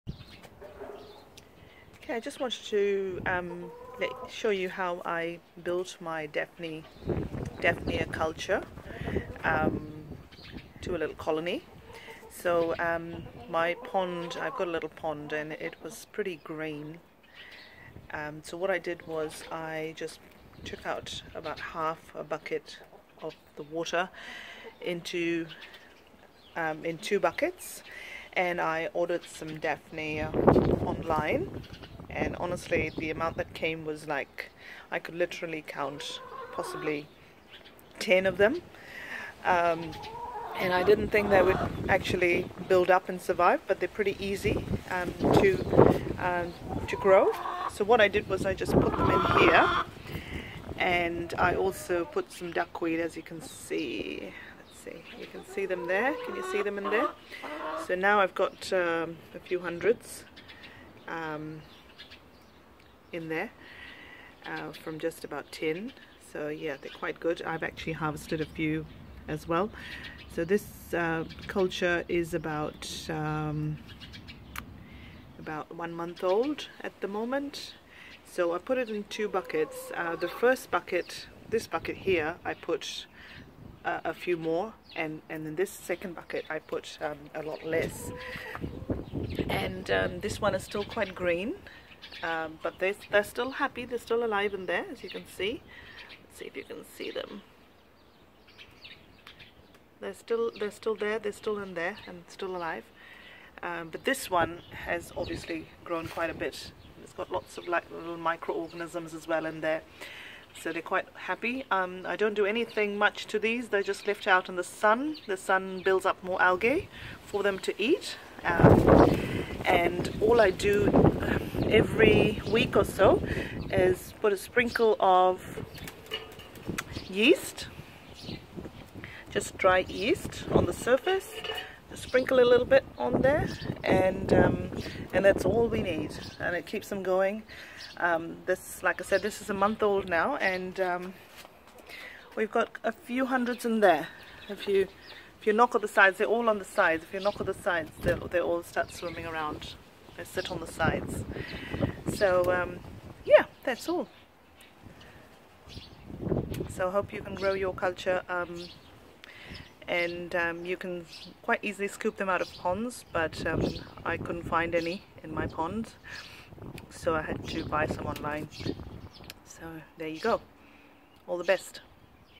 Easy how to multiply your Daphnia culture and build a healthy colony
Building a Daphnia colony was pretty easy - with just a few Daphnia to start with. These are tiny water fleas and the fish love them. They keep the pond water clean and provide a healthy diet for your fish.
This is what I did...surely there are other ways to do this!